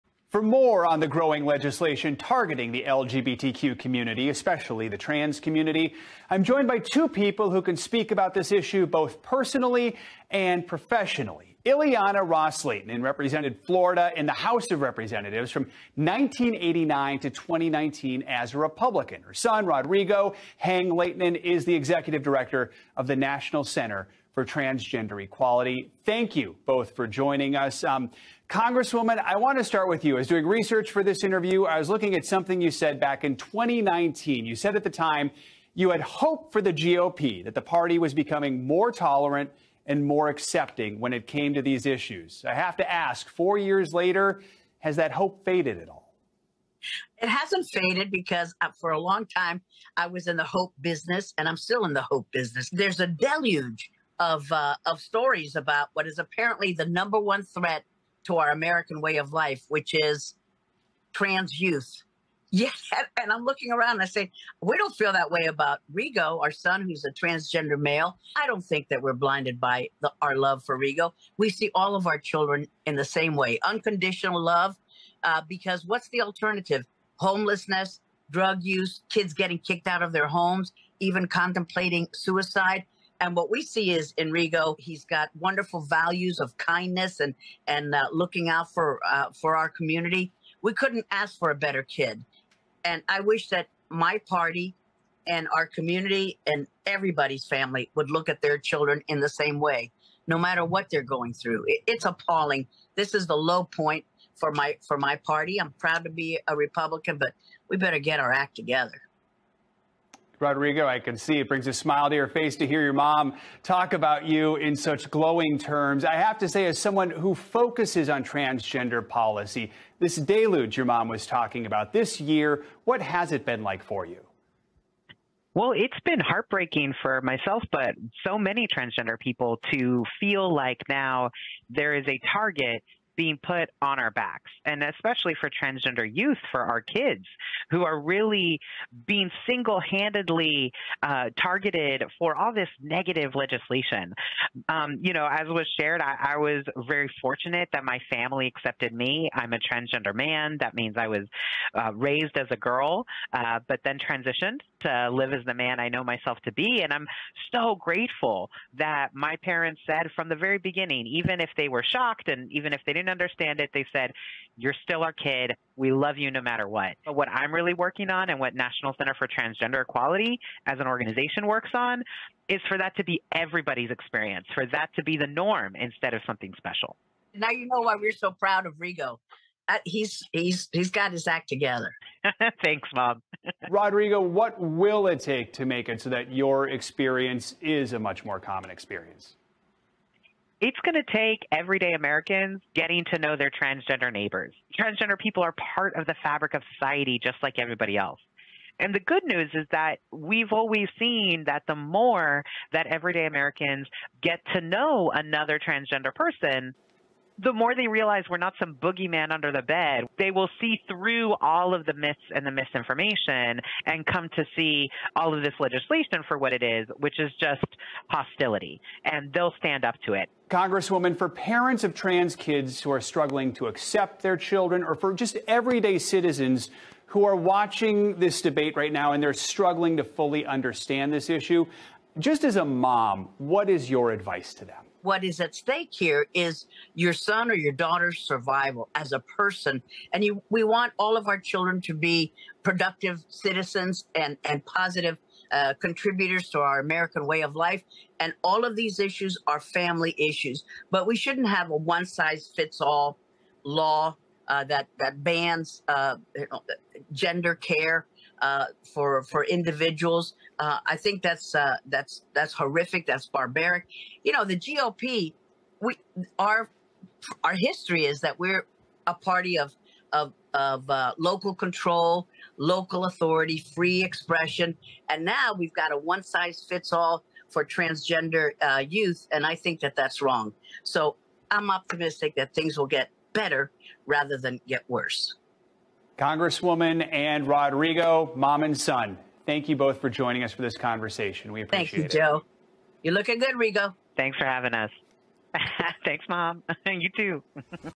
"We Love Our Children Unconditionally:" NCTE's Rodrigo Heng-Lehtinen appears on NBC with his mom!
As part of NBC's Pride programming, NCTE Executive Director Rodrigo Heng-Lehtinen appeared alongside his mom - former Congresswoman Ileana Ros-Lehtinen! - to discuss the importance of family support for trans kids. All children deserve to be loved for exactly who they are!